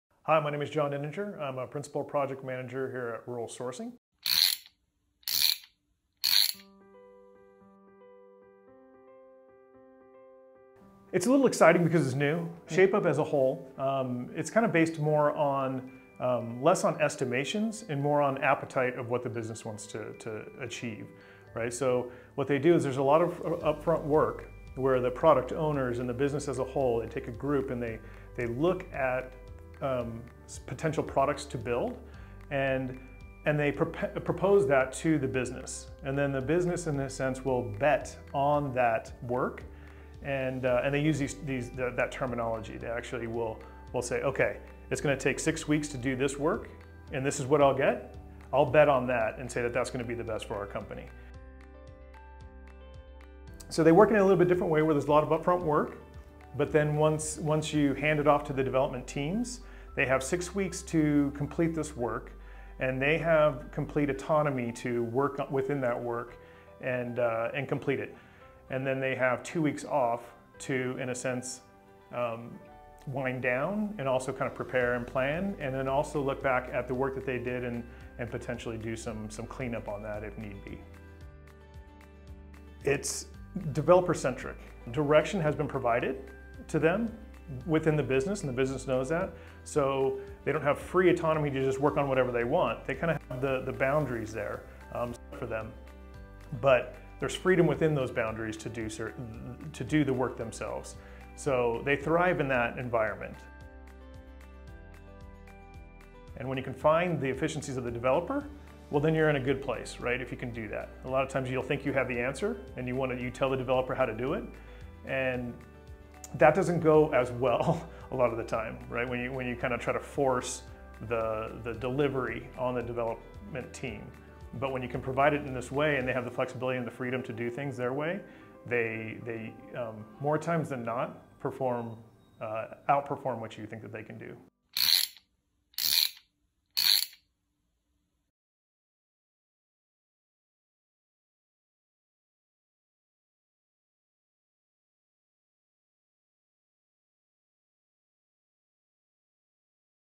Tech in 2: Shape Up
The Shape Up Method, an Agile software development approach, is helping address the risks and unknowns at each stage of product development in a more efficient and developer-centric manner. Principal Project Manager John Nenninger walks us through what makes this process different.